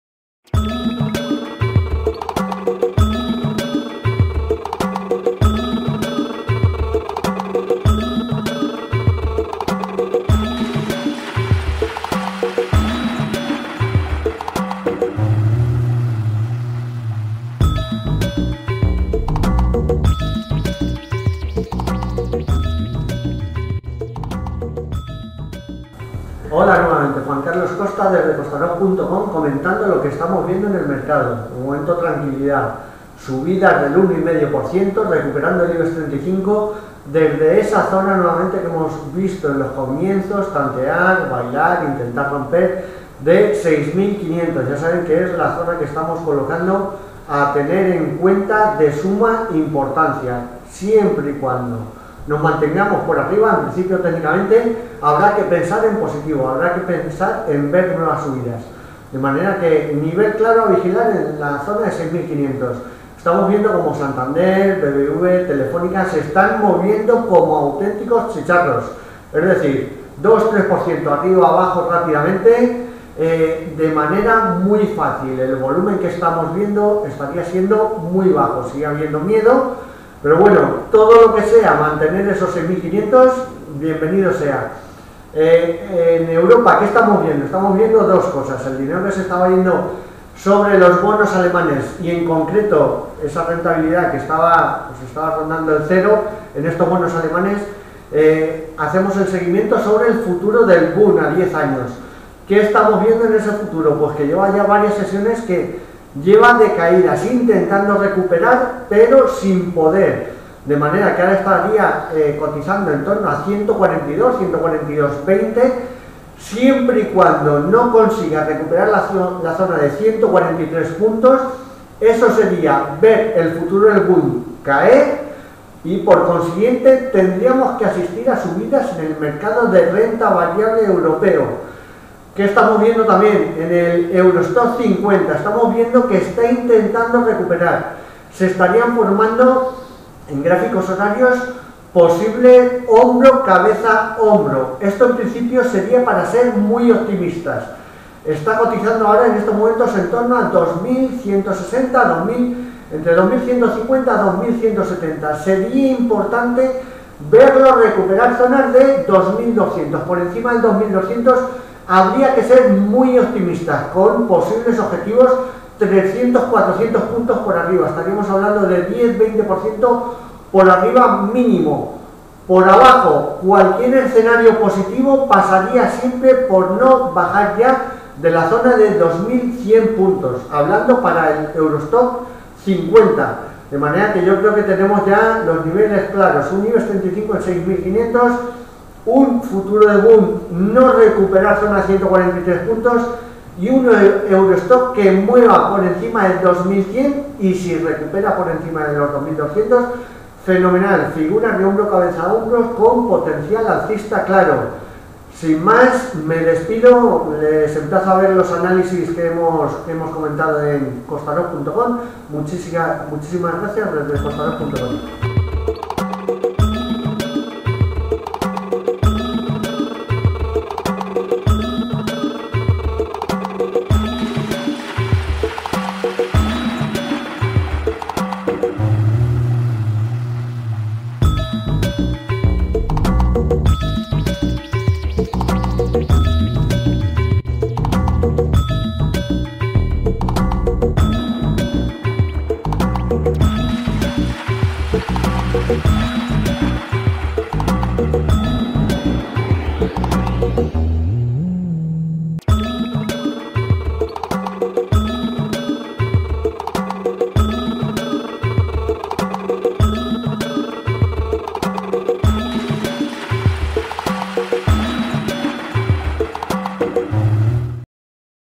Los mercados hoy (Kostarof Tv) 19/06/2012
Niveles claros a vigilar, vemos los blue chips moviéndose como chicharros, volúmenes por los suelos pero mientras mantengamos los 6500 calma. todos los análisis y video análisis